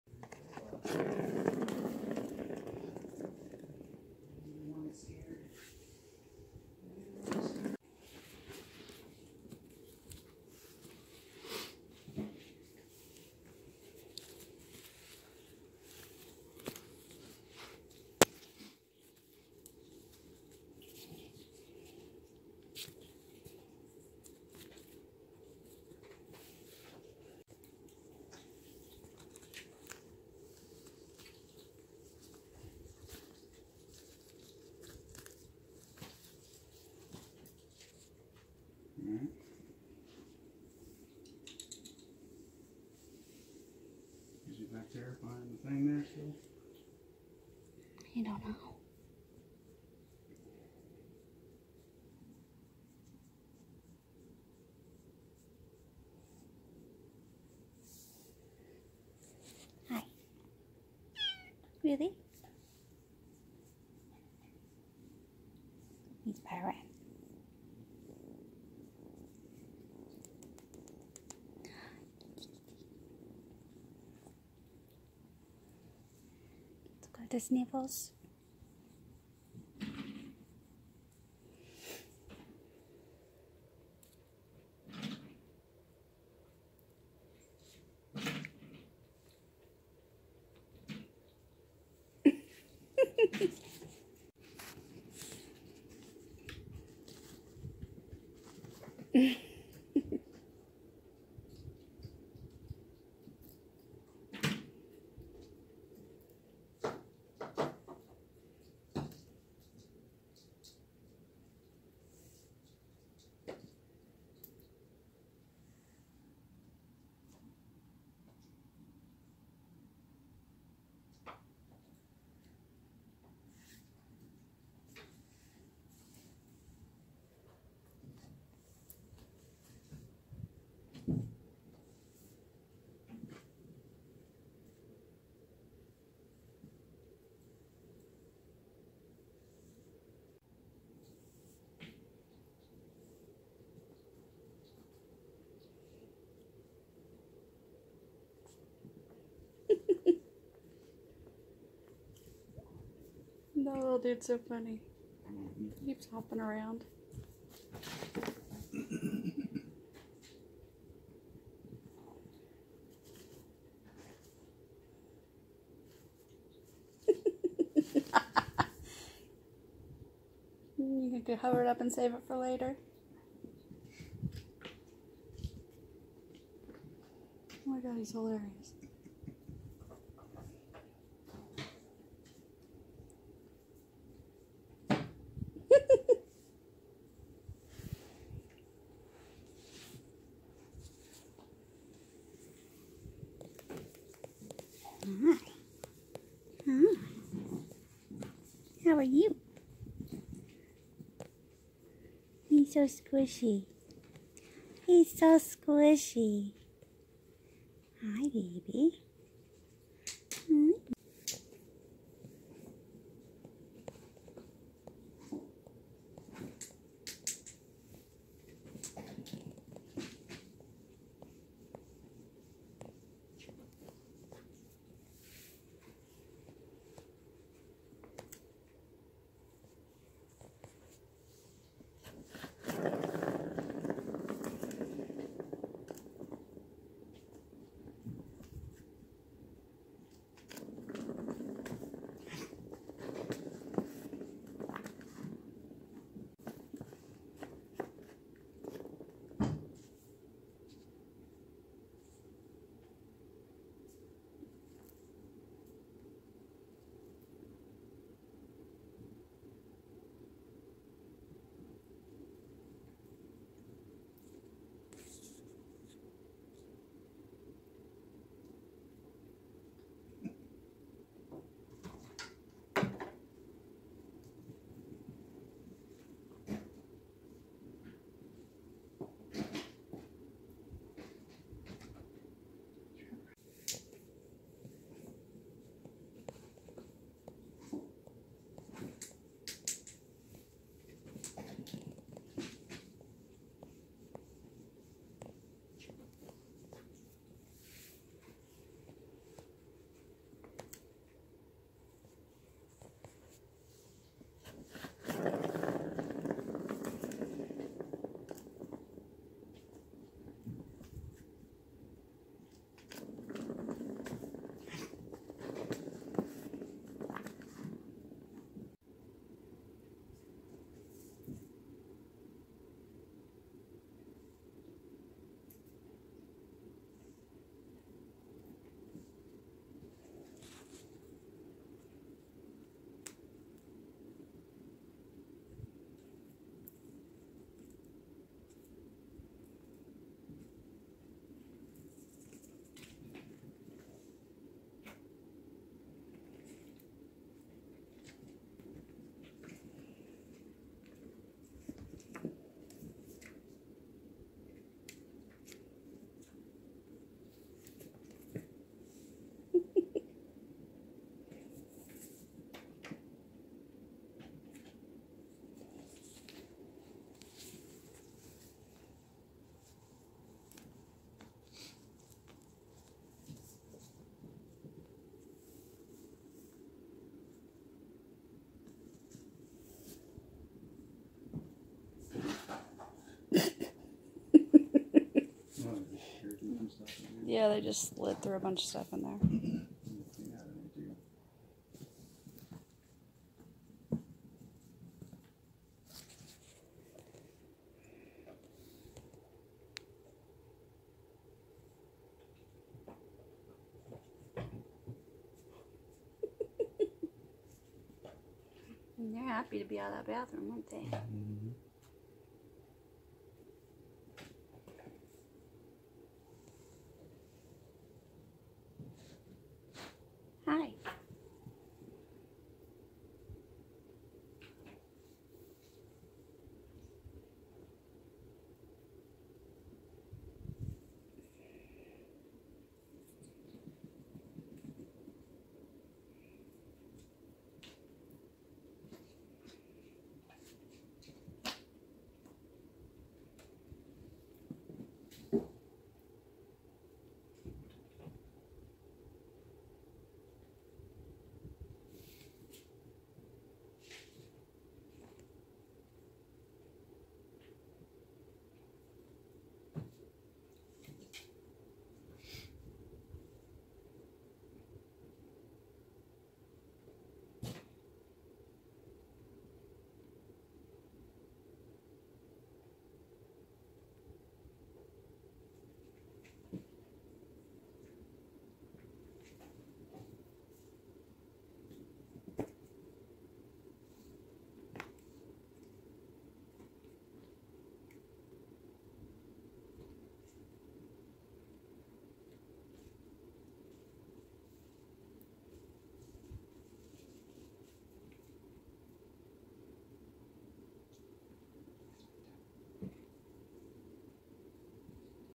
New Kittens Transport To A Rescue - Foster Litter #30
Litter #30 was chosen to be pulled by a rescue so we will only have them for a few days. Rescues will pull healthy, adoptable animals from over crowded shelters to give them a better chance at getting adopting. I was confused why they would want these guys since they are too young to adopt out right now so I asked and it turns out there is a kitty cold going around and most of the other cats in our shelter are too sick to be pulled and transported. So they are sending healthy babies and this will free up space and hopefully buy the sick kitties some more time. What happens is the animals that are chosen will be loaded up into a transport van and volunteers will drive the van from our Tulsa shelter to the rescue which I think in this case will be in another state. So this is a good thing for the animals.